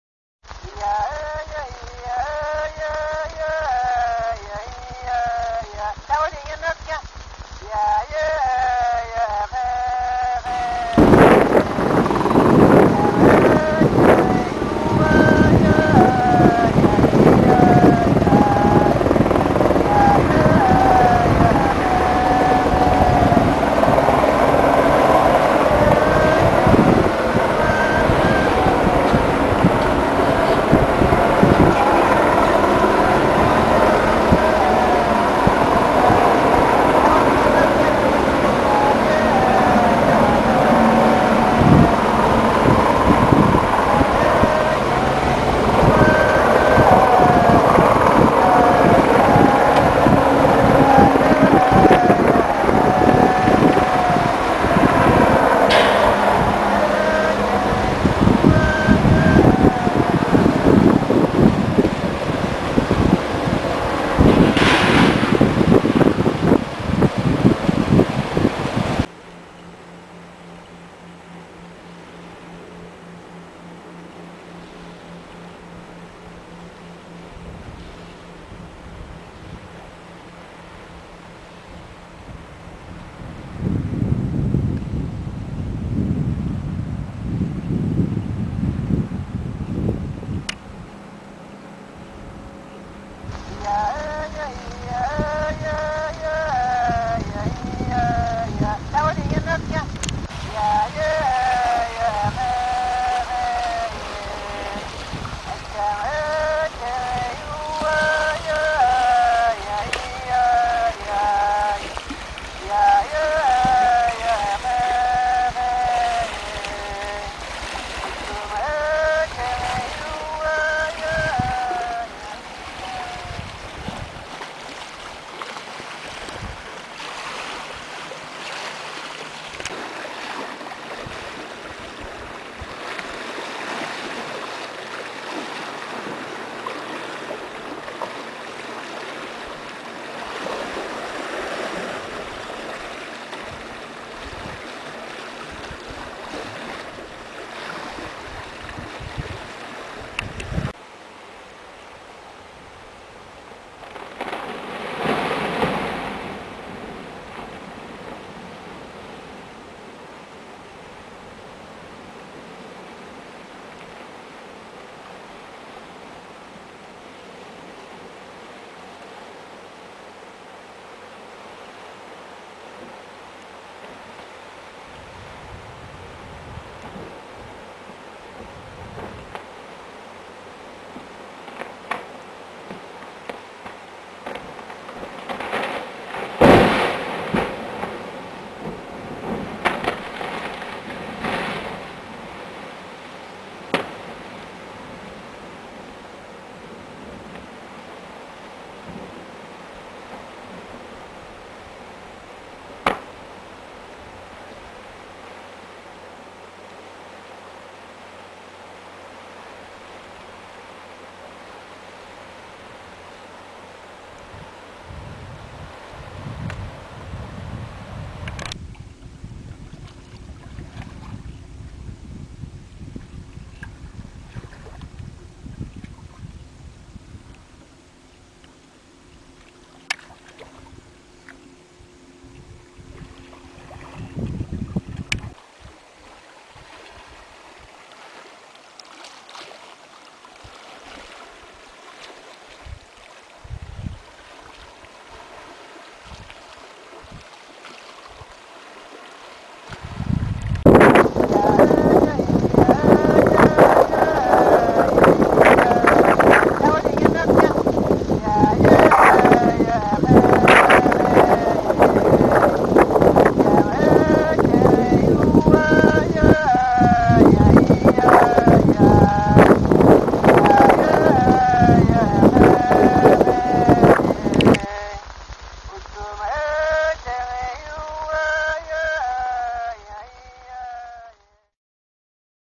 Salmon fishing in Greenland
Qaqortoq fishermen assist international efforts to assess Global climate affects on Atlantic salmon stocks. Though the Kalaallit Nunaat (Greenland) catch is small, it provides biologists with a unique insight to salmon migrating at great distances from their natal rivers